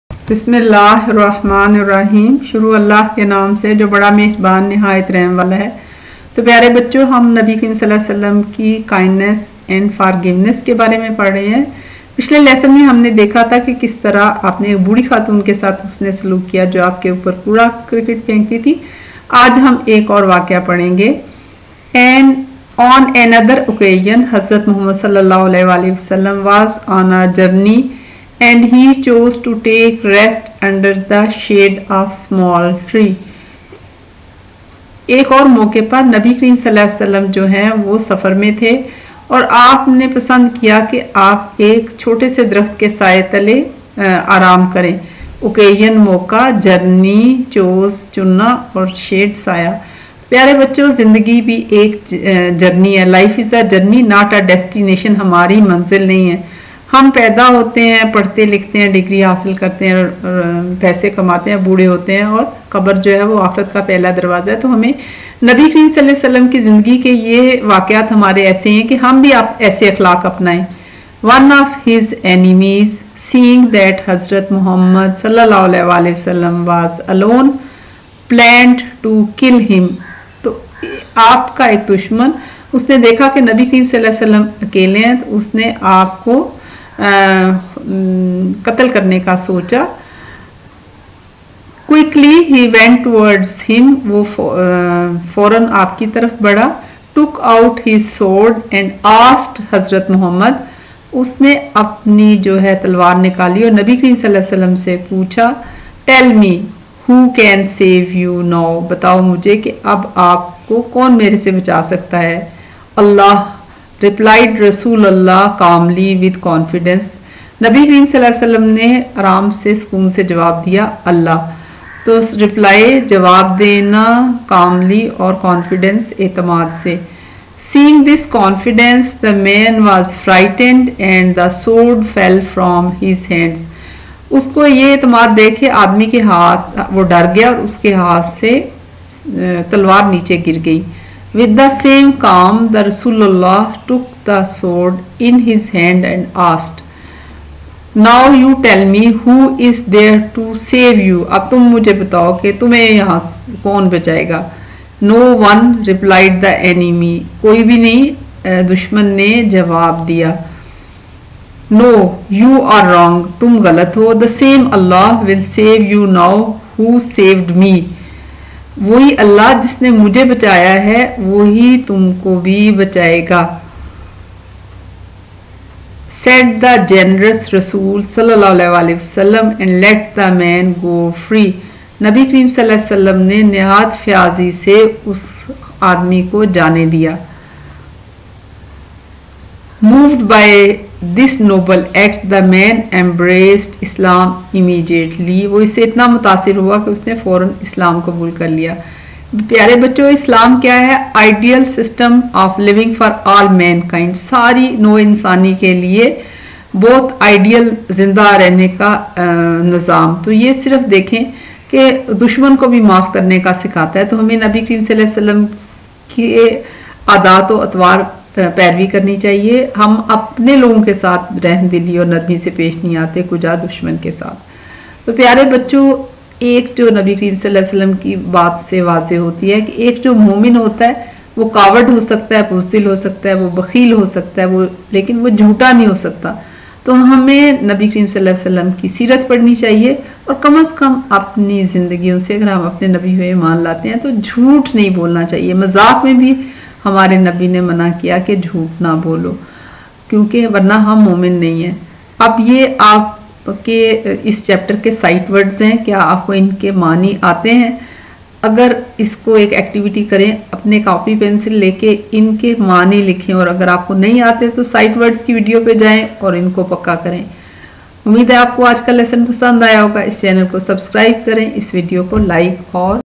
Hazrat Muhammad's Kindness and Forgiveness/English in Urdu
Chapter Hazrat Muhammad's kindness and forgiveness Lesson 3 for grade 5 kids